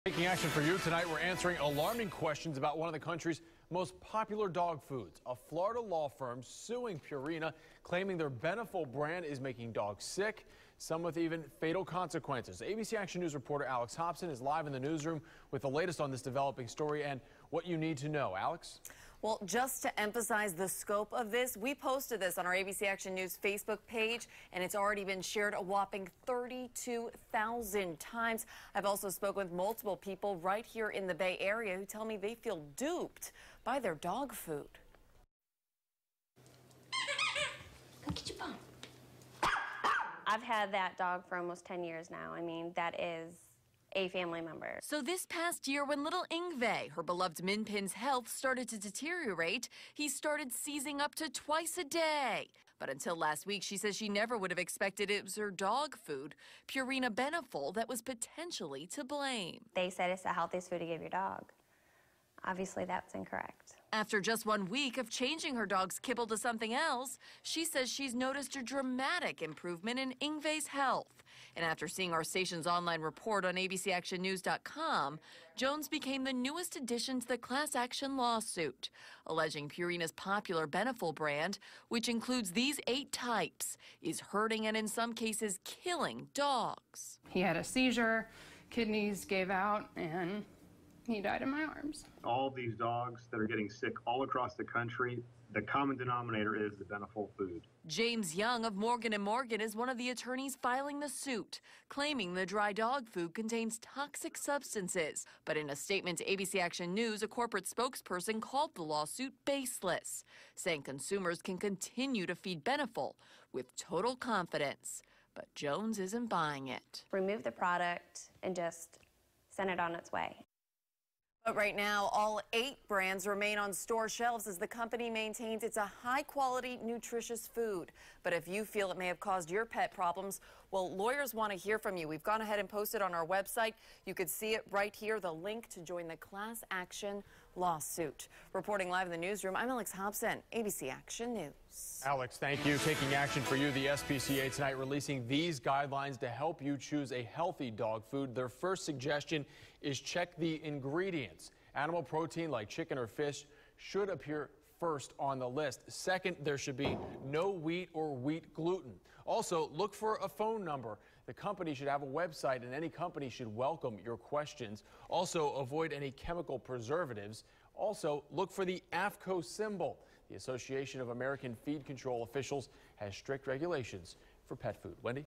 Purina defends Beneful dog food against claims
Purina is defending its Beneful brand pet food in the wake of a class action lawsuit that accuses the product of hurting and killing animals.

The suit claims that pet owner Frank Lucido’s three dogs became sick after he fed them the food. It alleges that Dozer, his English bulldog, died after showing signs of liver lesions and internal bleeding in his stomach.

One of the lawyers filing the suit, James Young of Morgan & Morgan in Tampa, is encouraging pet owners to come forward with their stories.

“The more people that come forward and share their stories, the clearer the picture we can paint with regard to the liability of the company,” Young said. “We received hundreds of complaints. Nationally, there have been thousands of complaints about dogs who have eaten this dog food by Purina Beneful and suffered very serious side effects, including death.”